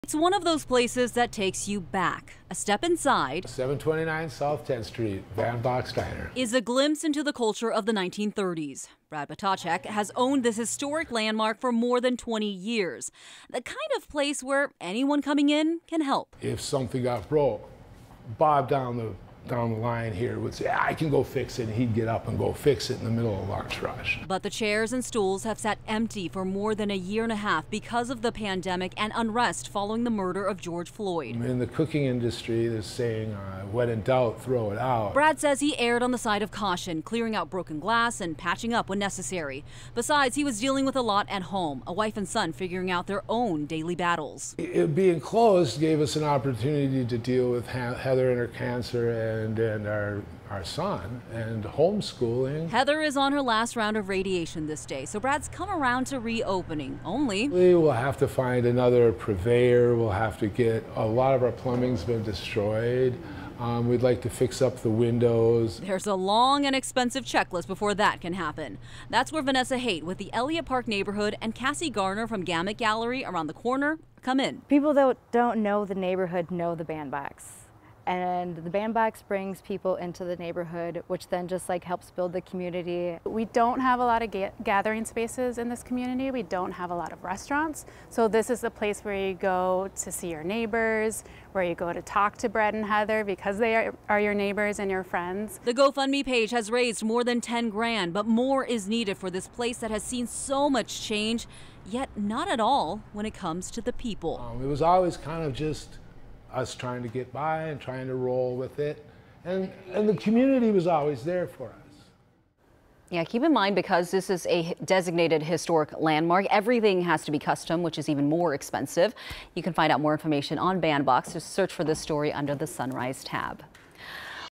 Minneapolis community comes together to raise funds to reopen iconic Band Box Diner
Owner Brad Ptacek closed his diner in Elliot Park at the beginning of the pandemic. He said there's an expensive checklist before he can reopen.